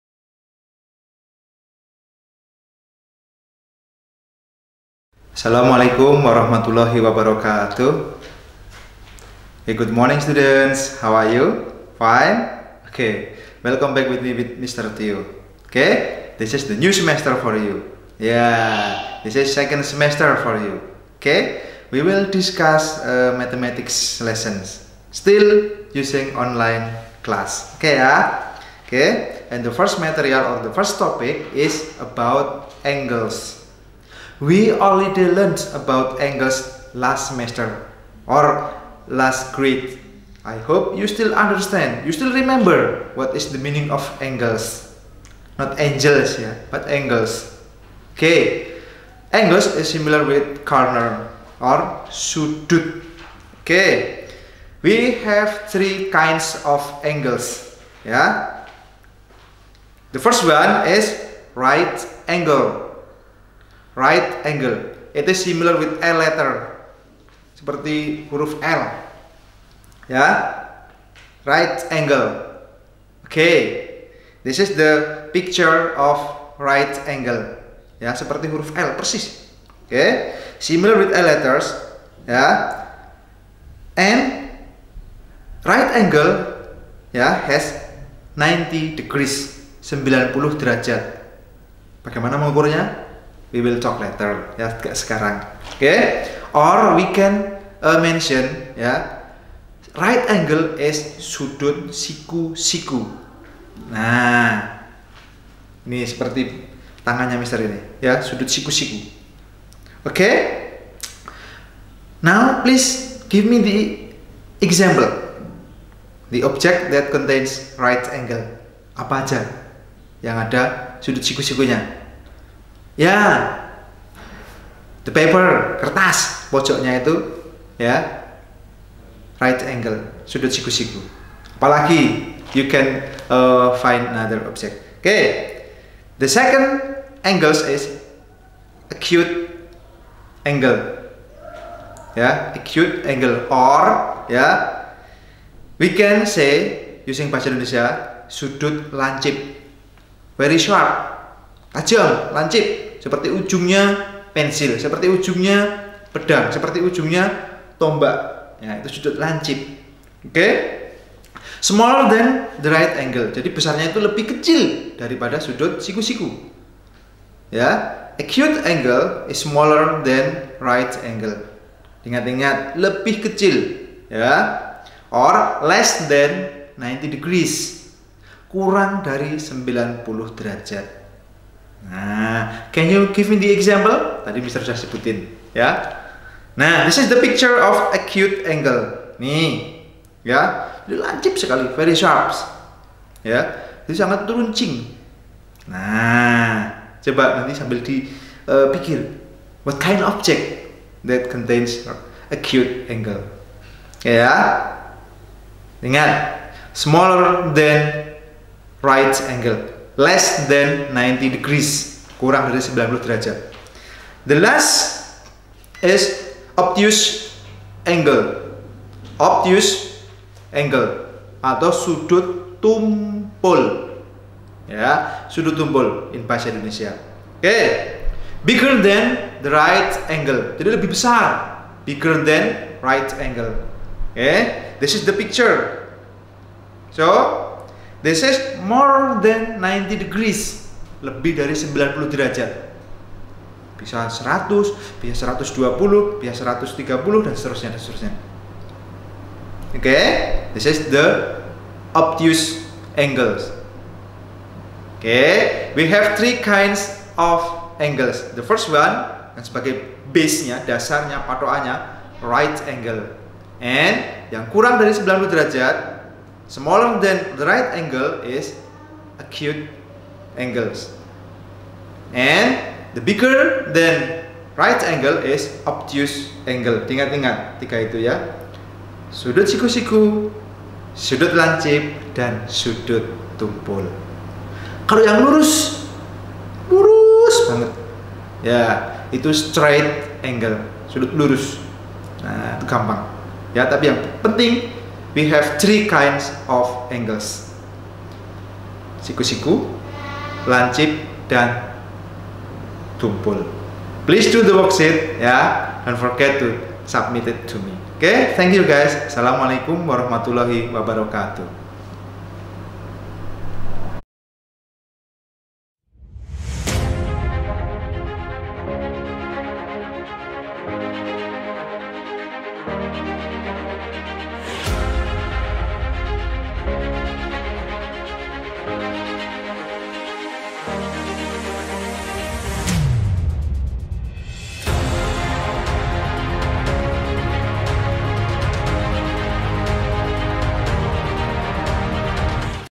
ANGLES | MATH GRADE 4
learning about the type of angles with Mr.Tyo